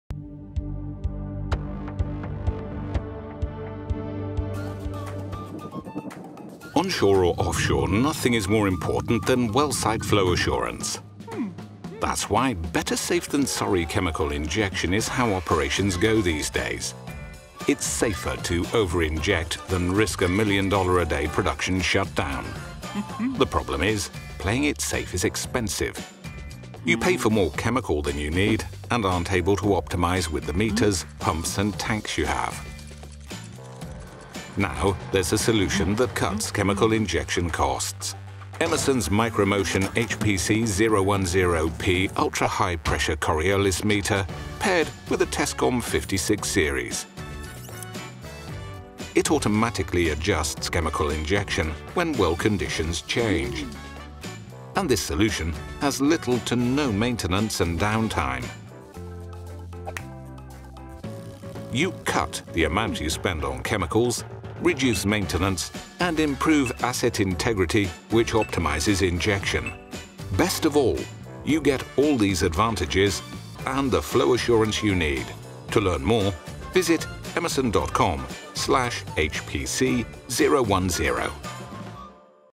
Micro Motion & TESCOM Chemical Injection Solutions
It is safer to over inject than risk a million a day production shutdown. The problem is that playing it safe is expensive. Emerson  Micro Motion HPC010P Ultra High-Pressure Coriolis Meter paired with TESCOM 56 Series Regulator automatically adjusts chemical injection with little to no maintenance and downtime.